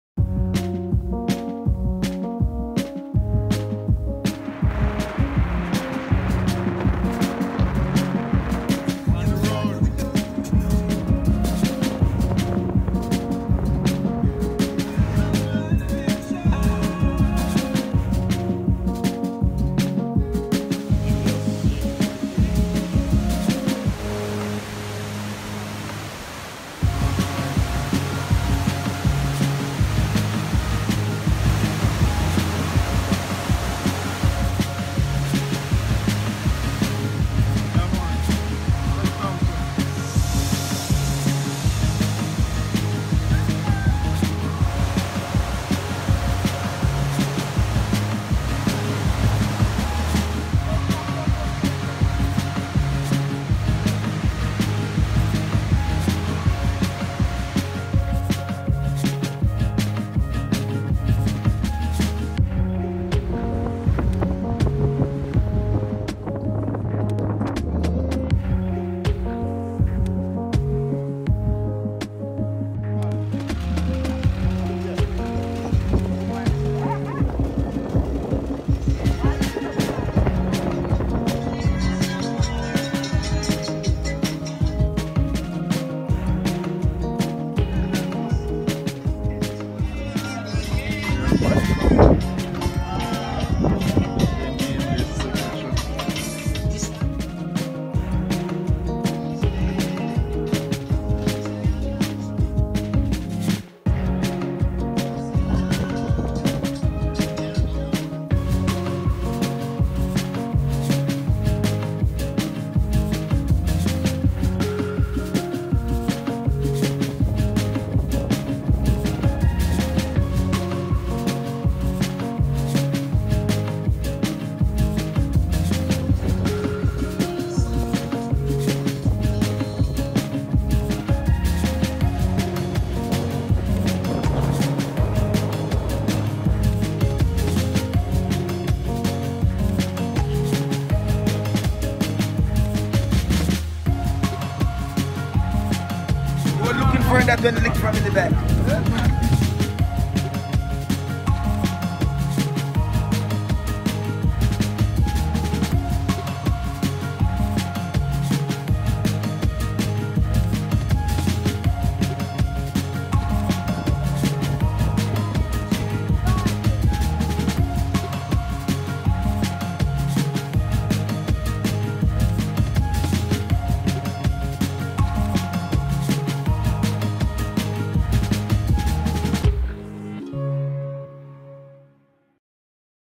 Webster tour-Road trip to Ys fall and the pelican bar with my guests
Road trip to Ys fall and the pelican bar with my guests